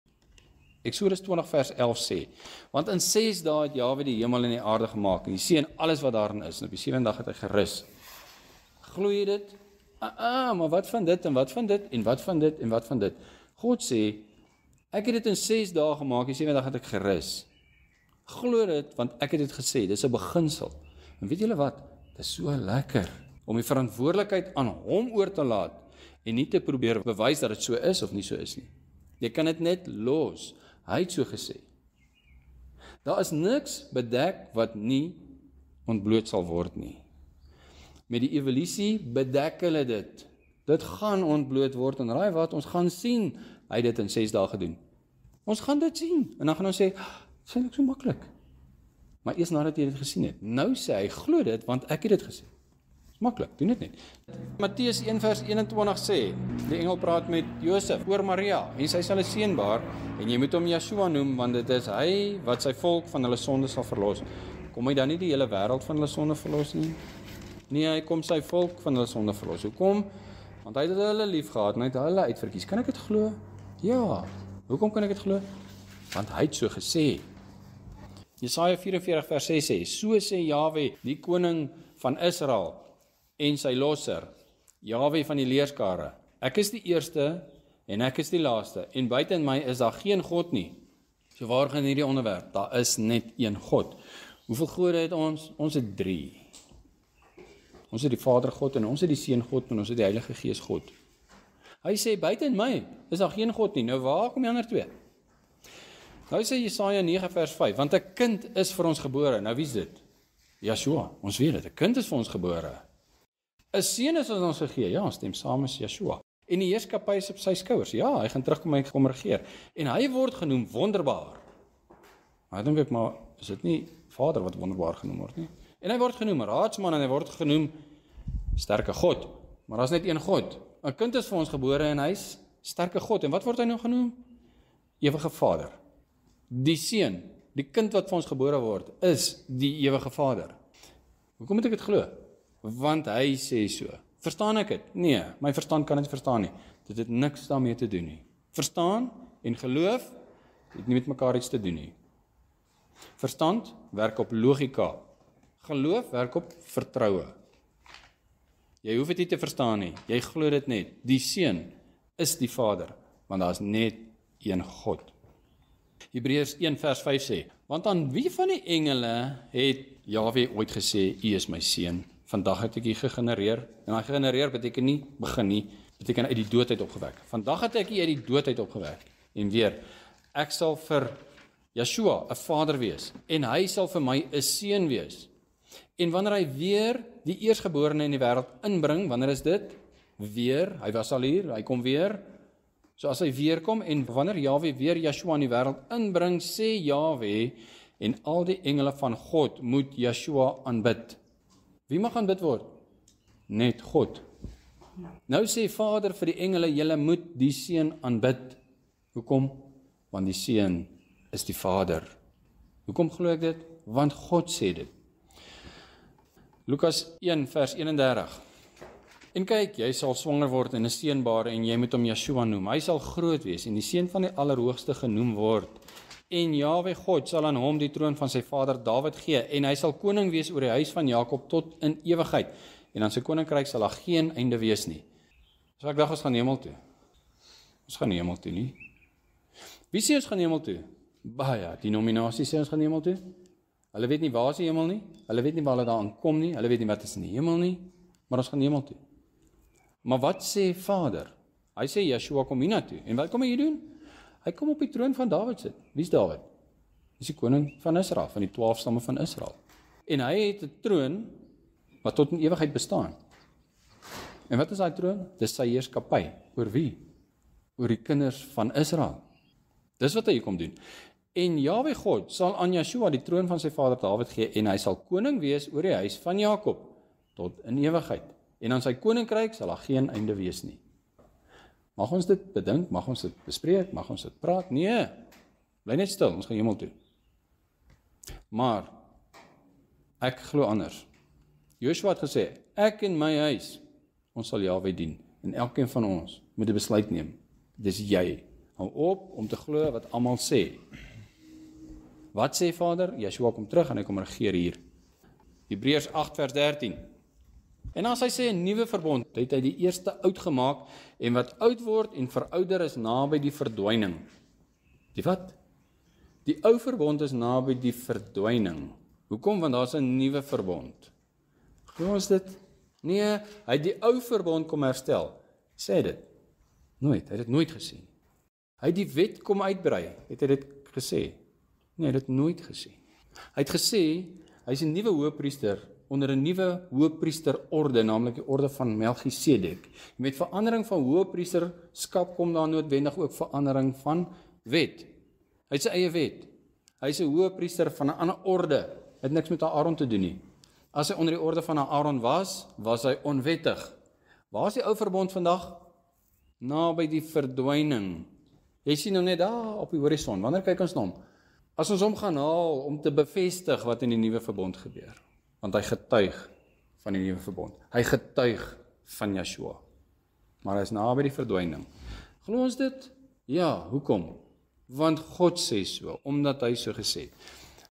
Jy kan dit net los, GOD het so gesê…
Exodus 20: 11  Want in ses dae het die HERE die hemel en die aarde gemaak, die see en alles wat daarin is, en op die sewende dag het Hy gerus. Daarom het die HERE die sabbatdag geseën en dit geheilig.
Deutoronómium 7: 6  Want jy is ‘n volk heilig aan die HERE jou God; jou het die HERE jou God uitverkies om uit al die volke wat op die aarde is, sy eiendomsvolk te wees. 7  Die HERE het ‘n welgevalle aan julle gehad en julle uitverkies, nie omdat julle meer was as al die ander volke nie, want julle was die geringste van al die volke. 8  Maar omdat die HERE julle liefgehad en die eed gehou het wat Hy vir julle vaders gesweer het, het die HERE julle met ‘n sterke hand uitgelei en jou uit die slawehuis, uit die hand van Farao, die koning van Egipte, verlos.
Matthéüs 1: 21  en sy sal ‘n seun baar, en jy moet Hom Jesus noem, want dit is Hy wat sy volk van hulle sondes sal verlos.
Jesaja 44: 6  So sê die HERE, die Koning van Israel en sy Losser, die HERE van die leërskare: Ek is die Eerste, en Ek is die Laaste, en buiten My is daar geen God nie.
Jesaja 9: 5  Want ‘n Kind is vir ons gebore, ‘n Seun is aan ons gegee; en die heerskappy is op sy skouer, en Hy word genoem: Wonderbaar, Raadsman, Sterke God, Ewige Vader, Vredevors—
Hebreërs 1: 5  Want aan wie van die engele het Hy ooit gesê: U is my Seun, vandag het Ek U gegenereer? en weer: Ek sal vir Hom ‘n Vader wees, en Hy sal vir My ‘n Seun wees? 6  En wanneer Hy weer die Eersgeborene in die wêreld inbring, sê Hy: En al die engele van God moet Hom aanbid.
Lukas 1: 31  En kyk, jy sal swanger word en ‘n Seun baar, en jy moet Hom Jesus noem. 32  Hy sal groot wees en die Seun van die Allerhoogste genoem word; en die Here God sal aan Hom die troon van sy vader Dawid gee, 33  en Hy sal koning wees oor die huis van Jakob tot in ewigheid, en aan sy koninkryk sal daar geen einde wees nie.
Josua 24: 15  Maar as dit verkeerd is in julle oë om die HERE te dien, kies dan vir julle vandag wie julle wil dien: òf die gode wat julle vaders daar oorkant die Eufraat gedien het, òf die gode van die Amoriete in wie se land julle woon; maar ek en my huis, ons sal die HERE dien.
Hebreërs 8: 13  As Hy sê ‘n nuwe verbond, het Hy die eerste oud gemaak; en wat oud word en verouder, is naby die verdwyning.